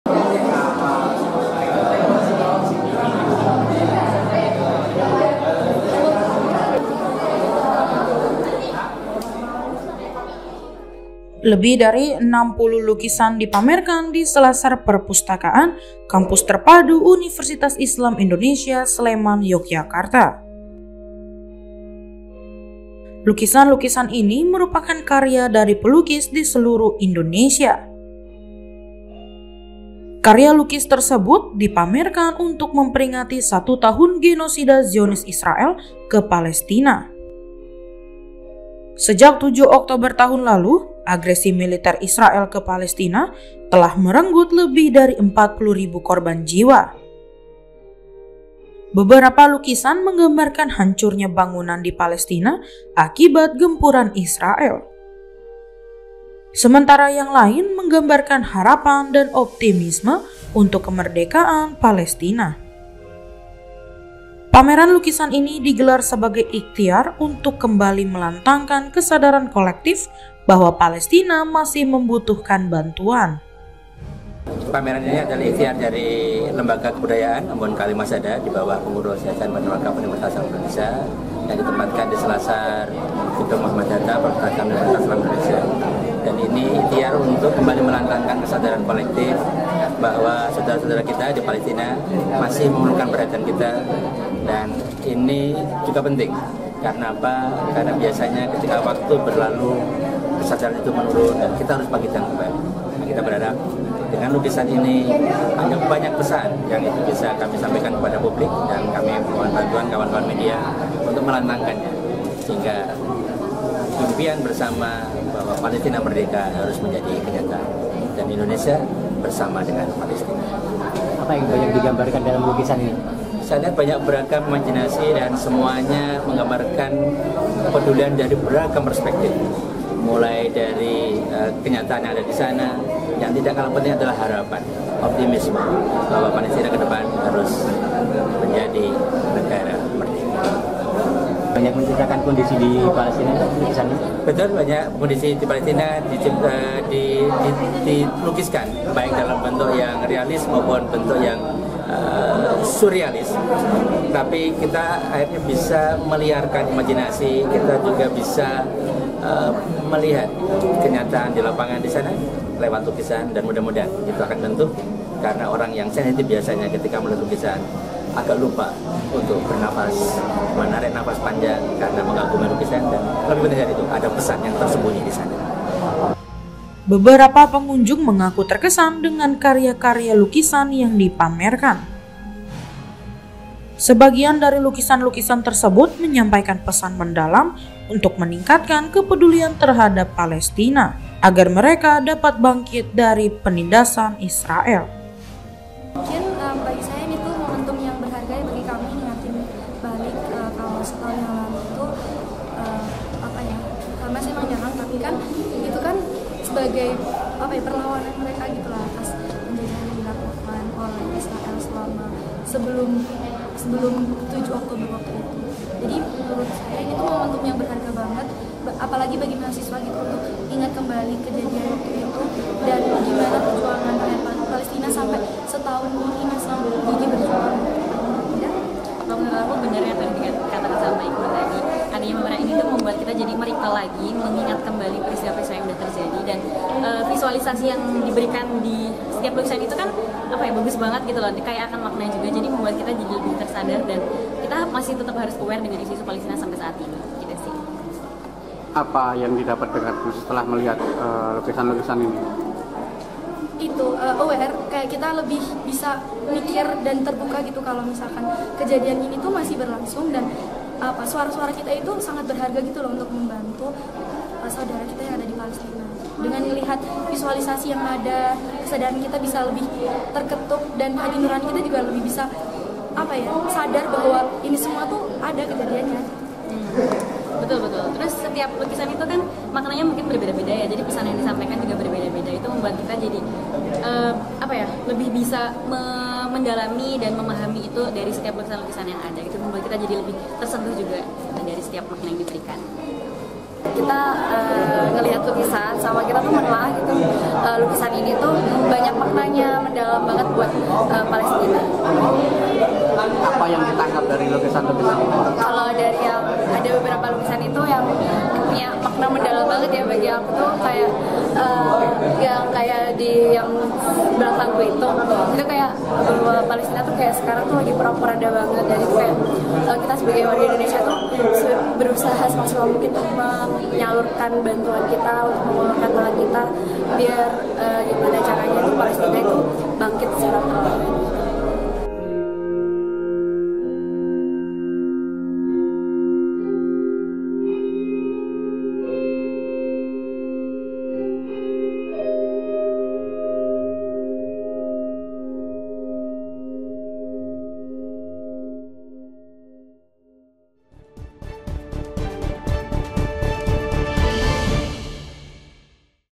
Respek! Puluhan Lukisan Bertema Palestina Dipamerkan di UII Yogyakarta | OneNewsUpdate
Jakarta, - Respek! Puluhan Lukisan Bertema Palestina Dipamerkan di UII Yogyakarta | OneNewsUpdate

Memperingati Setahun Genosida Israel ke Palestina, Pameran Lukisan Bertema Palestina Digelar di Yogyakarta Puluhan karya lukis dari berbagai penjuru Tanah Air dipamerkan secara gratis selama satu bulan. Pameran ini bertujuan mengingatkan kembali kekejaman Zionis Israel, sekaligus sebagai harapan agar bangsa Palestina segera merdeka. (AT01) (REG01) C026
NU01
ADT01